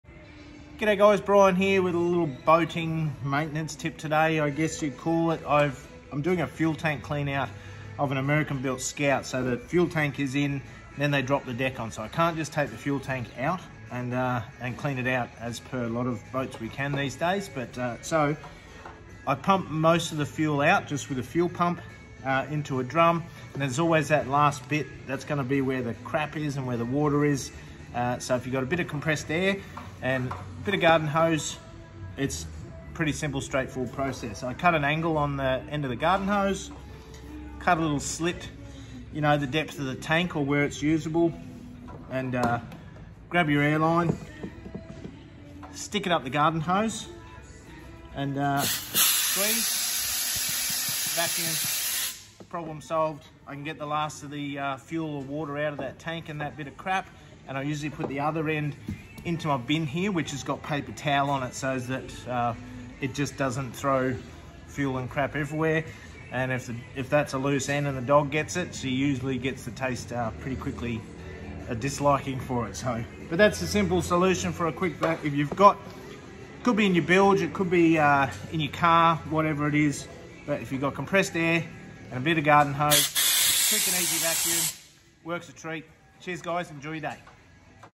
Boat maintenance tip when cleaning the bilge or a fuel tank and want to do it safely
Quick solution to sucking up debris out of your bilge or in this case a fuel tank and I didn’t want to use the vacuum cleaner for a number of reasons firstly being safety